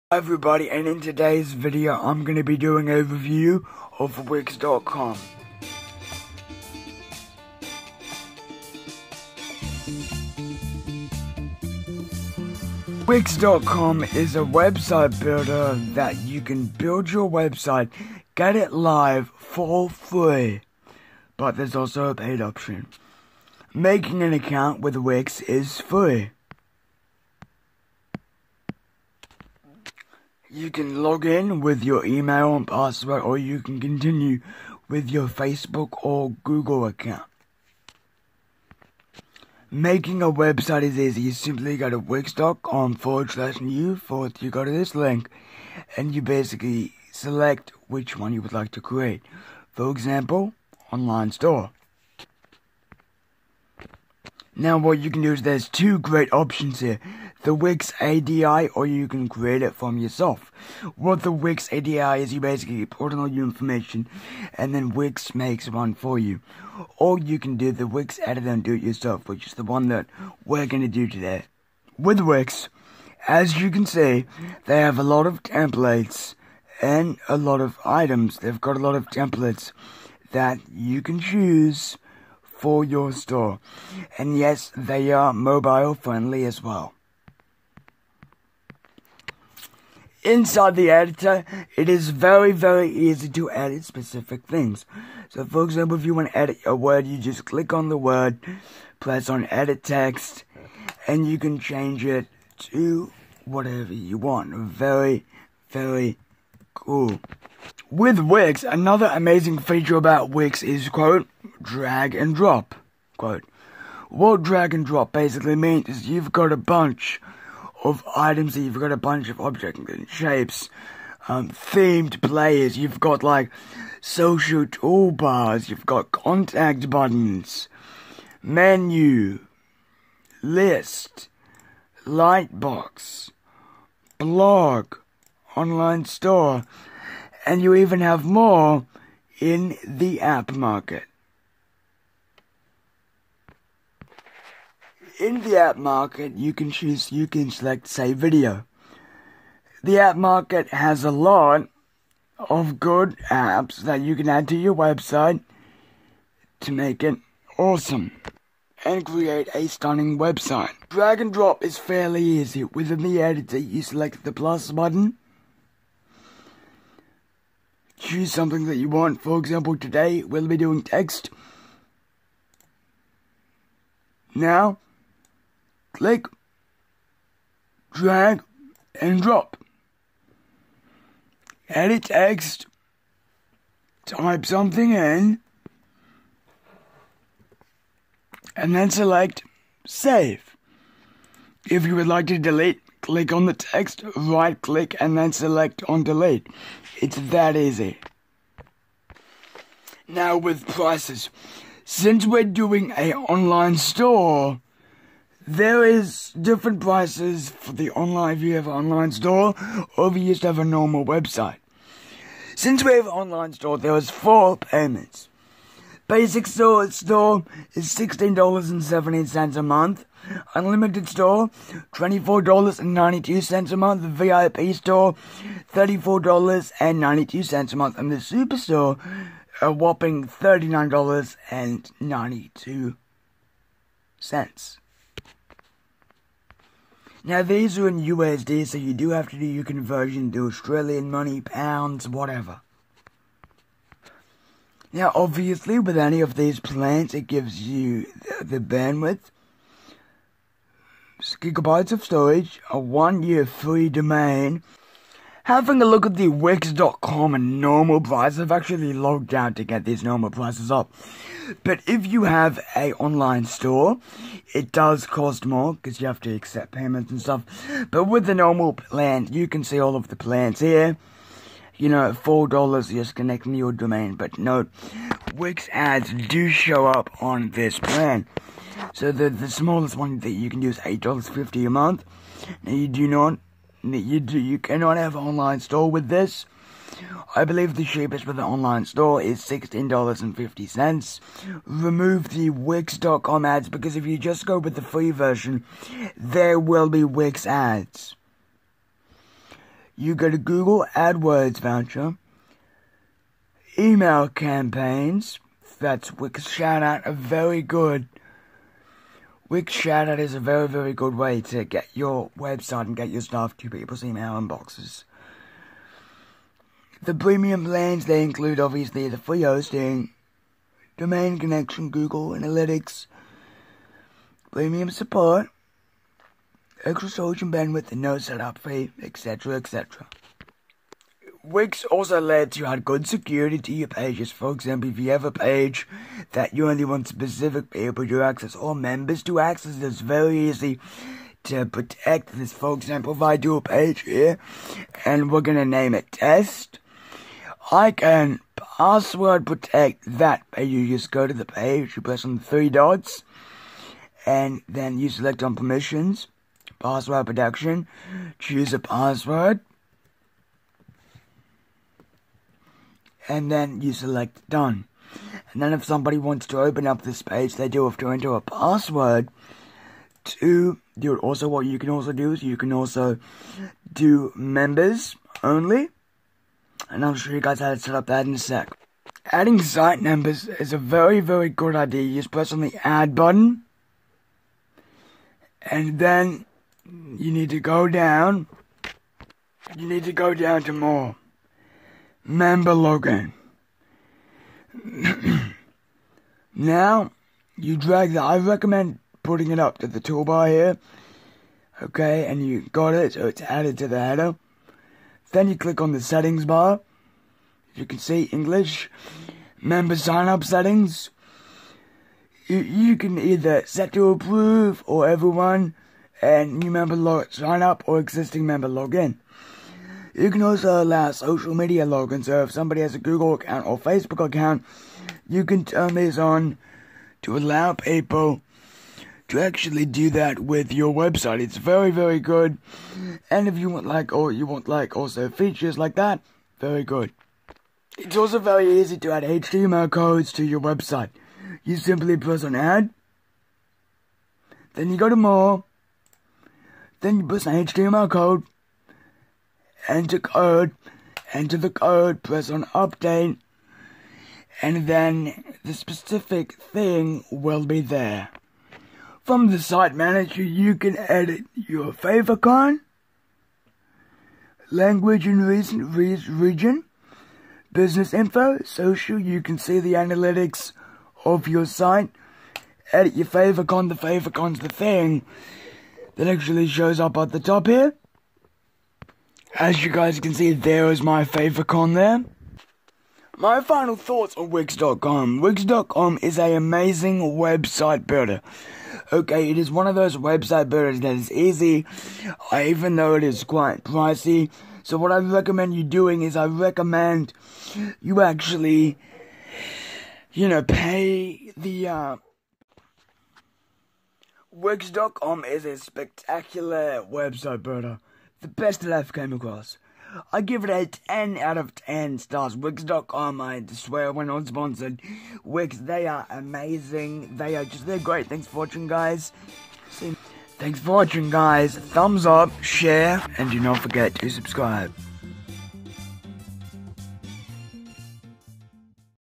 Wix.com website builder review - TECH TIME 57
Wix.com website builder review

Today we are doing a review of Wix.com a free website drag n drop builder.

Made by Kalab with the Soundcamp app.
The full track - © Kalab Network ©